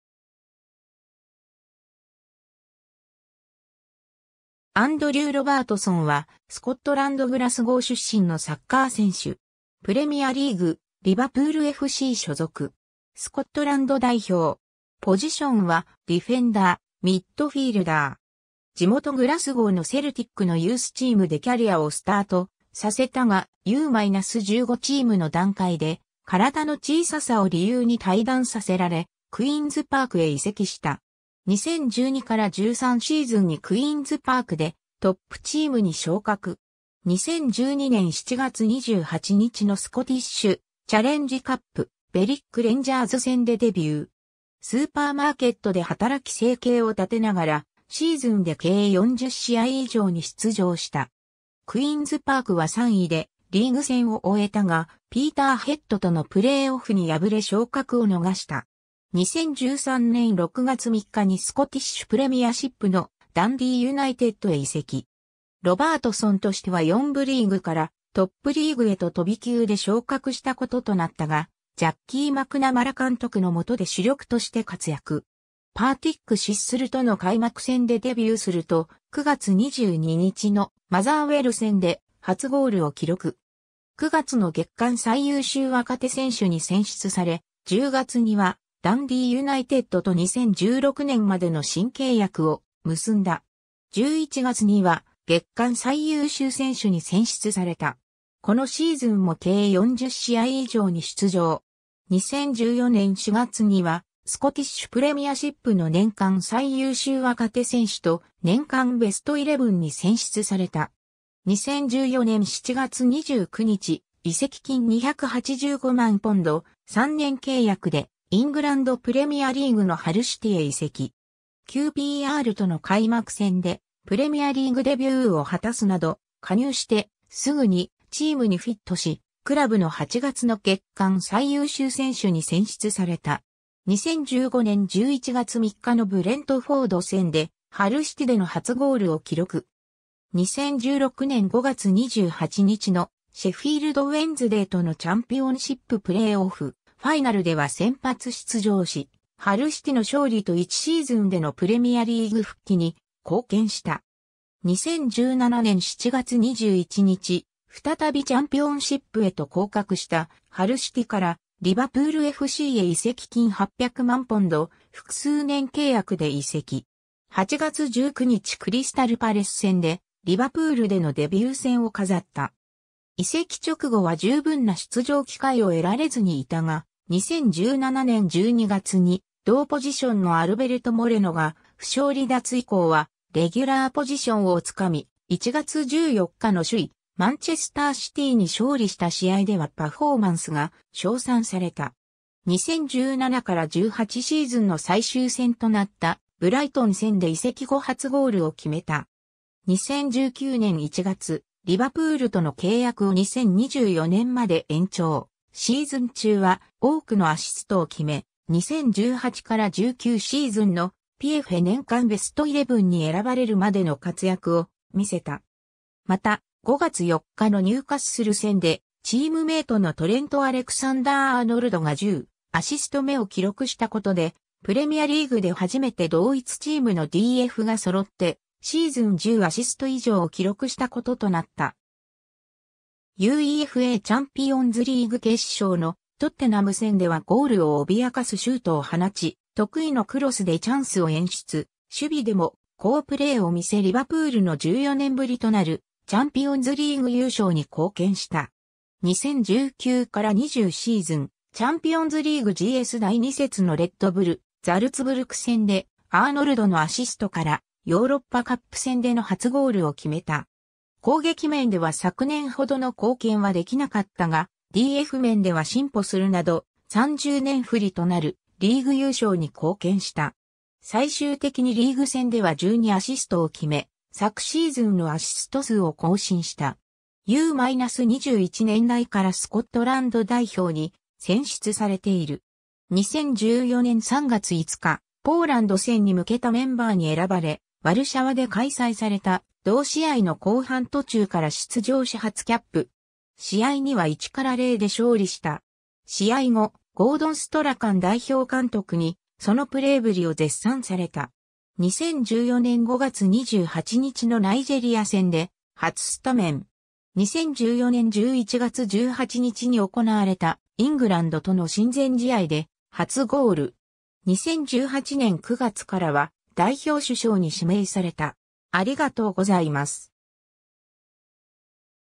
アンドリュー・ロバートソン (サッカー選手)
クイーンズ・パークFCの選手
ダンディー・ユナイテッドFCの選手
ハル・シティAFCの選手
アンドリュー・ロバートソン（Andrew Robertson 、1994年3月11日 - ）は、スコットランド・グラスゴー出身のサッカー選手。
プレミアリーグ・リヴァプールFC所属。
スコットランド代表。
ポジションはディフェンダー、ミッドフィールダー。
 地元グラスゴーのセルティックのユースチームでキャリアをスタートさせたが、U-15チームの段階で身体の小ささを理由に退団させられ、クイーンズ・パークへ移籍した。
 2012-13シーズンにクイーンズ・パーク（当時4部リーグ所属）でトップチームに昇格。
2012年7月28日のスコティッシュ・チャレンジカップ、ベリック・レンジャーズ戦でデビュー。
スーパーマーケットで働き生計を立てながら、シーズンで計40試合以上に出場した。
クイーンズ・パークは3位でリーグ戦を終えたが、ピーターヘッドとのプレーオフに敗れ昇格を逃した。
 2013年6月3日にスコティッシュ・プレミアシップのダンディー・ユナイテッドへ移籍。
ロバートソンとしては4部リーグからトップリーグへと飛び級で昇格したこととなったが、ジャッキー・マクナマラ監督の元で主力として活躍。
パーティック・シッスルとの開幕戦でデビューすると、9月22日のマザーウェル戦で初ゴールを記録。
9月の月間最優秀若手選手に選出され、10月にはダンディー・ユナイテッドと2016年までの新契約を結んだ。
11月には月間最優秀選手に選出された。
このシーズンも計40試合以上に出場。
2014年4月にはスコティッシュ・プレミアシップの年間最優秀若手選手と年間ベストイレブンに選出された。
 2014年7月29日、移籍金285万ポンド、3年契約でイングランド・プレミアリーグのハル・シティへ移籍。
QPRとの開幕戦でプレミアリーグデビューを果たすなど、加入してすぐにチームにフィットし、クラブの8月の月間最優秀選手に選出された。
 2015年11月3日のブレントフォード戦でハル・シティでの初ゴールを記録。
2016年5月28日のシェフィールド・ウェンズデイとのチャンピオンシップ・プレーオフ・ファイナルでは先発出場し、ハル・シティの勝利と1シーズンでのプレミアリーグ復帰に貢献した。
 2017年7月21日、再びチャンピオンシップへと降格したハル・シティからリヴァプールFCへ移籍金800万ポンド、複数年契約で移籍。
8月19日クリスタル・パレス戦でリヴァプールでのデビュー戦を飾った。
 移籍直後は十分な出場機会を得られずにいたが、2017年12月に同ポジションのアルベルト・モレノが負傷離脱以降はレギュラーポジションを掴み、1月14日の首位マンチェスター・シティに勝利した試合ではパフォーマンスが称賛された。
2017-18シーズンの最終戦となったブライトン戦で移籍後初ゴールを決めた。
 2019年1月、リヴァプールとの契約を2024年まで延長、シーズン中は多くのアシストを決め、2018-19シーズンのPFA年間ベストイレブンに選ばれるまでの活躍を見せた。
また、5月4日のニューカッスル戦でチームメートのトレント・アレクサンダー＝アーノルドが10アシスト目を記録したことで（この時点で既にロバートソンは10アシストを記録していた）、プレミアリーグで初めて同一チームのDFが揃ってシーズン10アシスト以上を記録したこととなった（ロバートソン自身は11アシストを記録した）。
 UEFAチャンピオンズリーグ決勝のトッテナム戦ではゴールを脅かすシュートを放ち、得意のクロスでチャンスを演出、守備でも好プレーを見せリヴァプールの14年ぶりとなるチャンピオンズリーグ優勝に貢献した。
 2019-20シーズン、チャンピオンズリーグGS第2節のレッドブル・ザルツブルク戦でアーノルドのアシストからヨーロッパカップ戦での初ゴールを決めた。
攻撃面では昨年程の貢献は出来なかったが、DF面では進歩するなど、30年振りとなるリーグ優勝に貢献した。
最終的にリーグ戦では12アシストを決め、昨シーズンのアシスト数を更新した。
 U-21年代からスコットランド代表に選出されている。
 2014年3月5日、ポーランド戦に向けたメンバーに選ばれ...